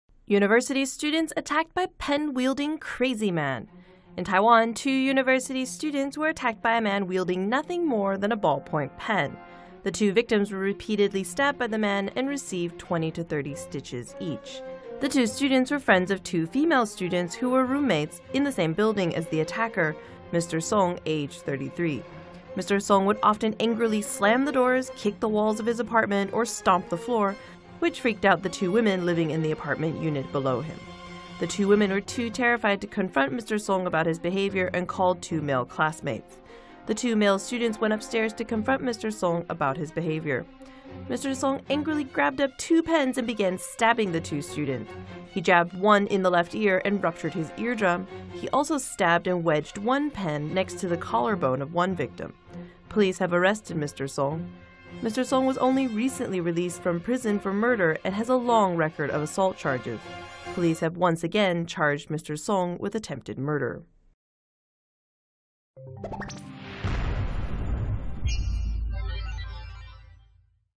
Recently released murderer attacks two university students with pens - TomoNews
PINGTUNG, TAIWAN - In Taiwan, two university students were attacked by a man wielding nothing more than a ballpoint pen. The two victims were repeatedly stabbed by the man and received 20-30 stitches each.

The two students were friends of two female students who were roommates in the same building as the attacker, Mr. Song, age 33.

Mr. Song would often angrily slam the doors, kick the walls of his apartment, or stomp the floor which freaked out the two women living in the apartment unit below him. The two women were too terrified to confront Mr. Song about his behavior and called two male classmates.

The two male students went upstairs to confront Mr. Song about his behavior. Mr. Song angrily grabbed up two pens and began stabbing the two students. He jabbed one in the left ear and ruptured his eardrum, he also stabbed and wedged one pen next to the collarbone of one victim.

Police have arrested Mr. Song. Mr. Song was only recently released from prison for murder and has a long record of assault charges. Police have once again charged Mr. Song with attempted murder.